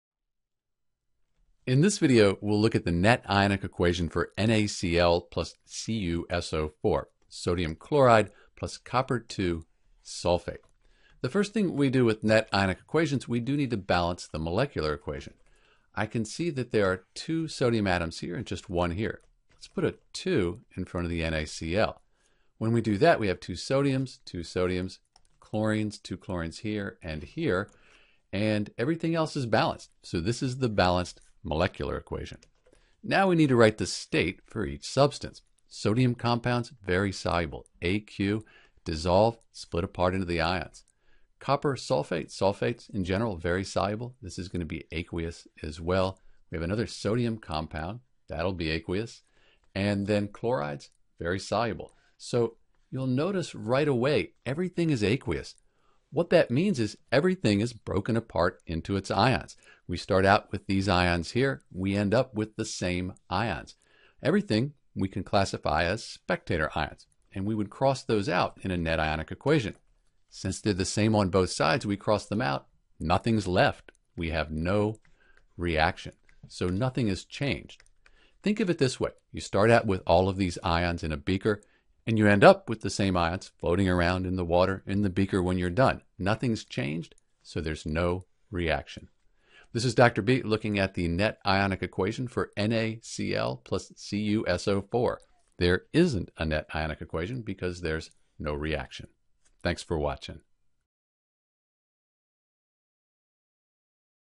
How to Write the Net Ionic Equation for NaCl + CuSO4 = Na2SO4 + CuCl2
There are three main steps for writing the net ionic equation for NaCl + CuSO4 = Na2SO4 + CuCl2 (Sodium chloride + Copper (II) sulfate). First, we balance the molecular equation.

Because all of the compounds are soluble (aq), all of the ions are spectator ions. No precipitate (or gas or liquid) will form and therefore NO REACTION has taken place.  If a precipitate did form, we would not split the compound apart into ions.

Important Skills

More Practice

General Steps:

1. Write the balanced molecular equation.
2. Write the state (s, l, g, aq) for each substance.
3. Split soluble compounds into ions (the complete ionic equation).
4. Cross out the spectator ions on both sides of complete ionic equation.
5. Write the remaining substances as the net ionic equation.